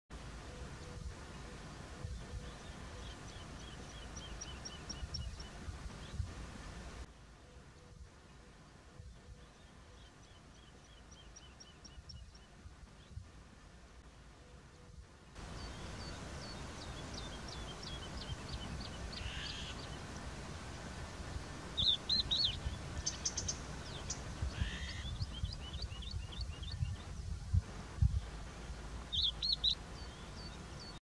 Curruca Mosquitera y Phytolaca Americana
La Curruca Mosquitera está en plena migración, aprovecha los frutos de la planta alóctona Phytolaca Americana para recuperar fuerzas